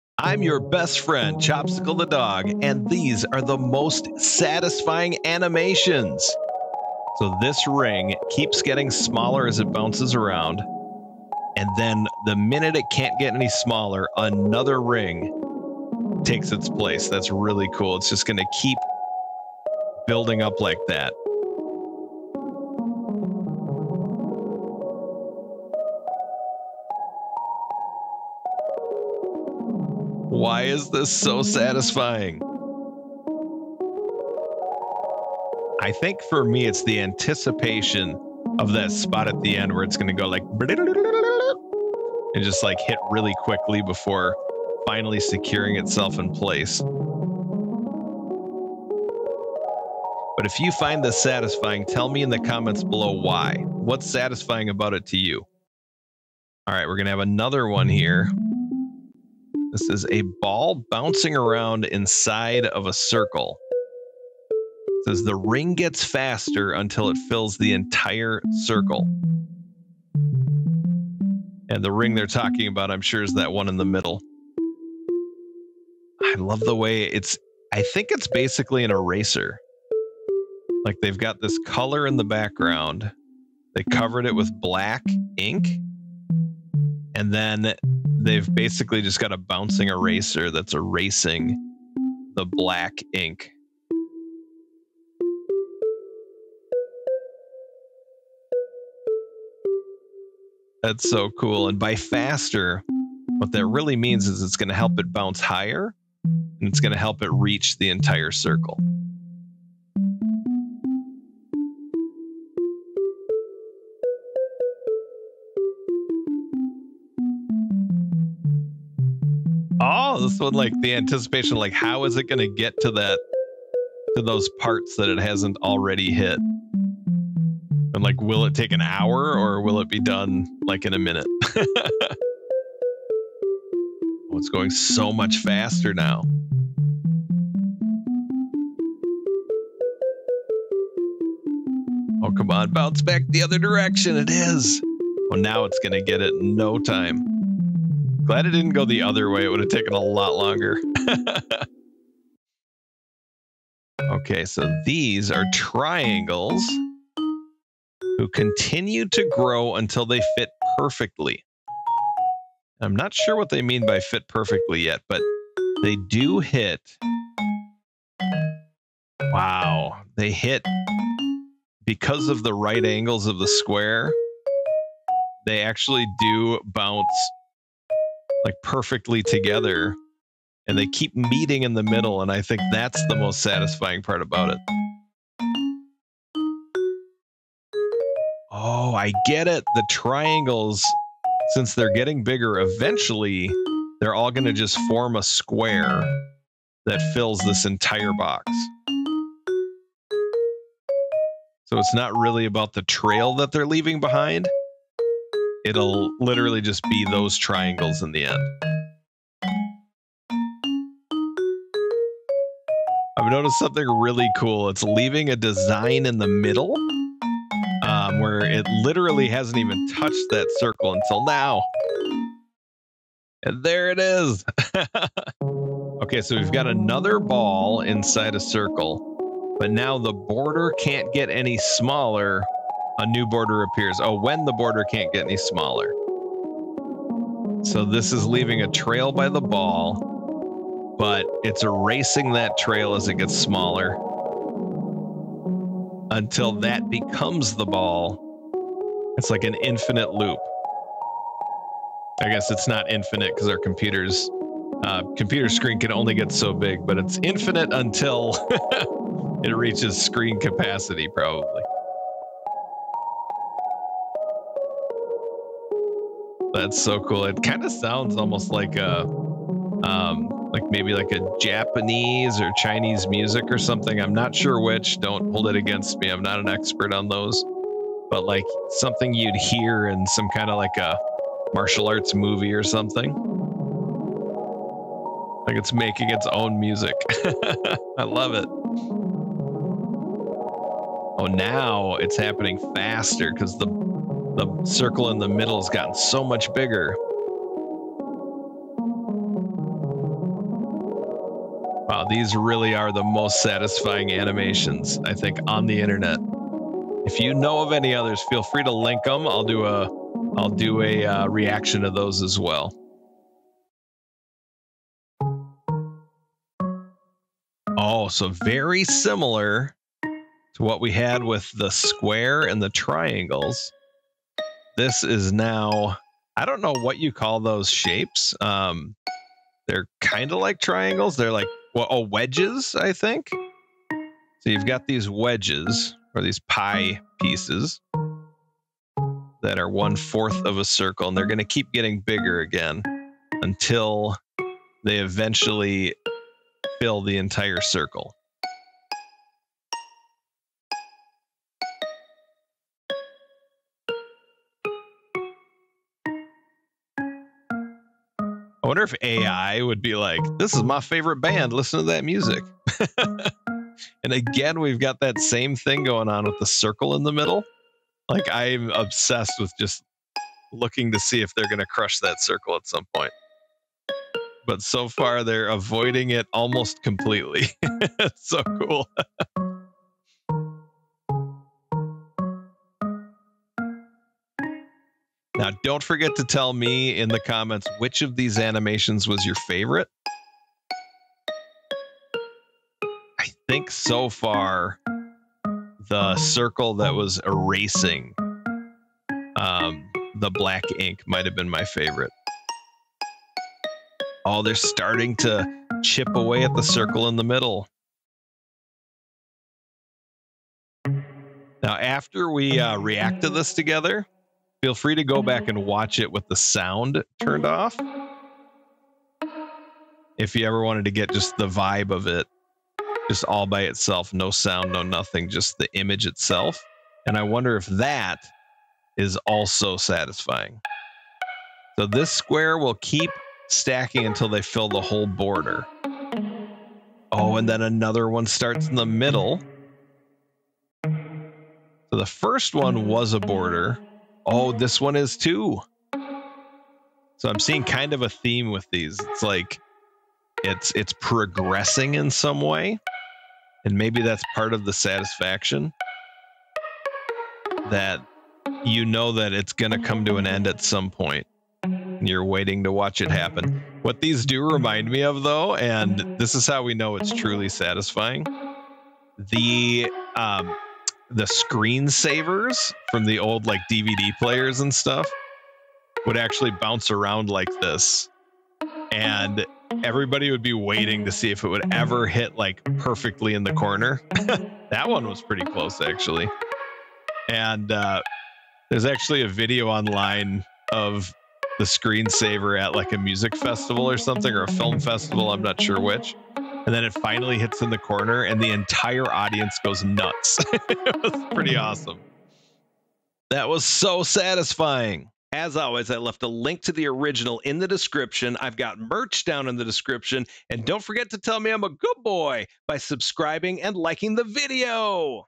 Top 5 Most Satisfying Asmr Animations +1 - (Reaction)
These are the Top 5 Best ASMR Animations on the internet! These animations combine sight and sound to create the most satisfying animations you could see! Watch in awe as a circle bounces inside another circle making music as it bounces! The circle itself gets smaller changing the cadence of the sound until they unite in the most satisfying way possible! These animations are ASMR at it's finest!

I'm Chospicle The Dog and this is my reaction to the Most Satisfying ASMR Animations on the internet!